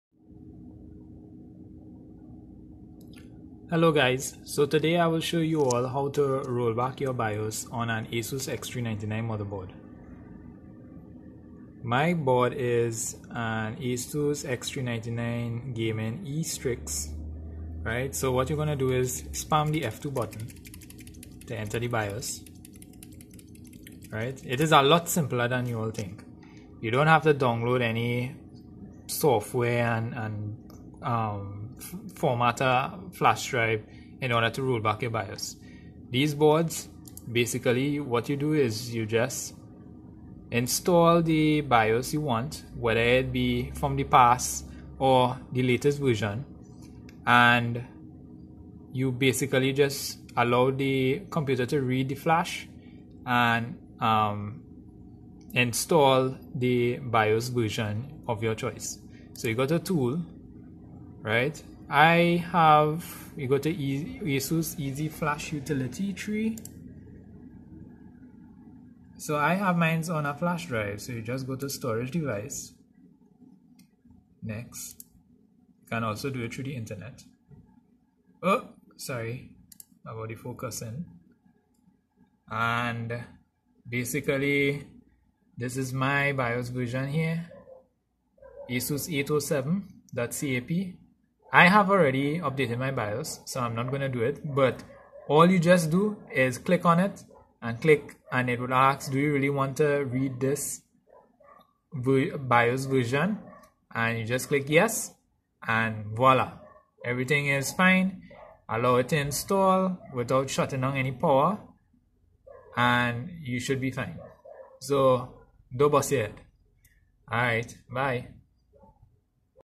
How to Rollback your Bios on an Asus X399 Motherboard, Hint: Easier than you think
It is exactly the same process of installing an updated BIOS for these motherboard. No formatting of flash and downloading Afudos necessary.